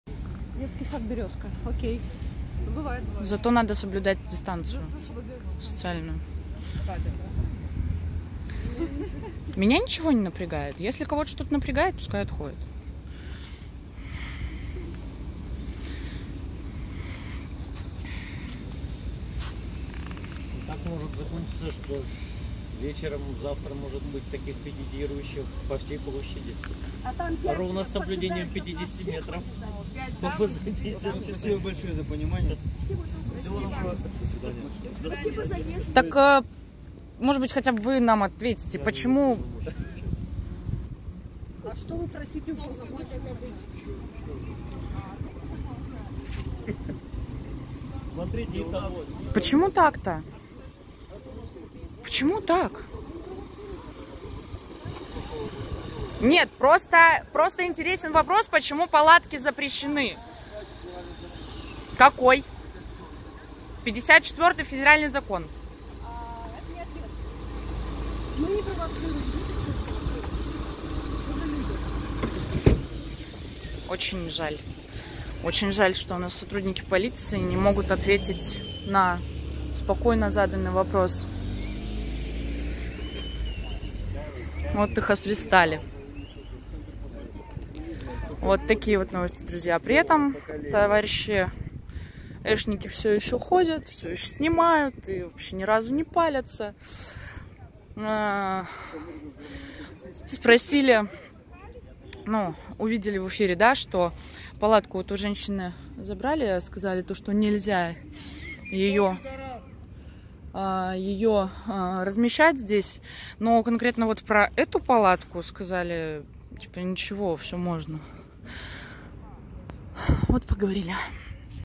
ПЕРВАЯ ПАЛАТКА В ХАБАРОВСКЕ на площади!
Сотрудники полиции изъяли у женщины палатку, но не смогли объяснить, зачем это сделали.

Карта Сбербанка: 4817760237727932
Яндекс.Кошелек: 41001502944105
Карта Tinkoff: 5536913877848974